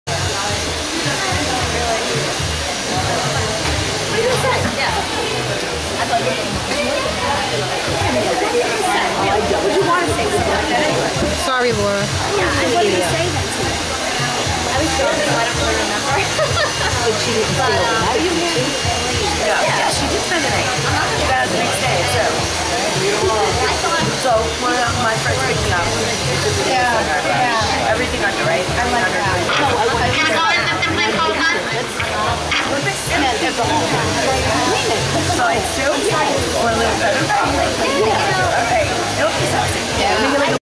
Michelle Clunie, Melanie on Queer as Folk, visits Metromodes Salon/Spa Bellmore, NY
Michelle Clunie visits Metromodes Salon/Spa in Bellmore, NY.  Hairstyling by Sebastian